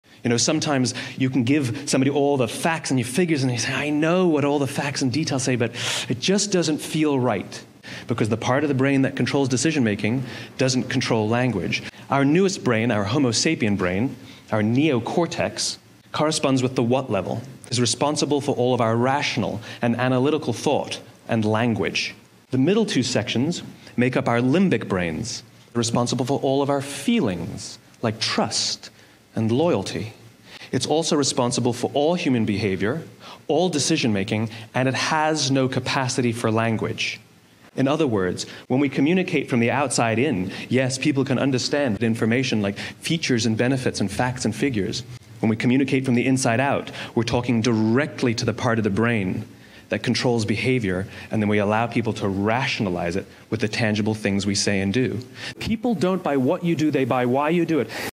How to communicate to the REAL decision maker (Part of the brain 🧠)
"I know what all the facts and figures say but it just doesn't feel right."

The part of the brain (The REAL decision maker) that controls all behavior doesn't really care about the facts and figures. Learn how to drive behaviors and decisions making from Simon Sinek, best selling author of "Start with Why".

"People don't buy what you do, people buy WHY you do what you do"

Want to learn what we do in Northern Lights Studio? We help brands build a sustainable competitive advantage in an era full of me-too offerings.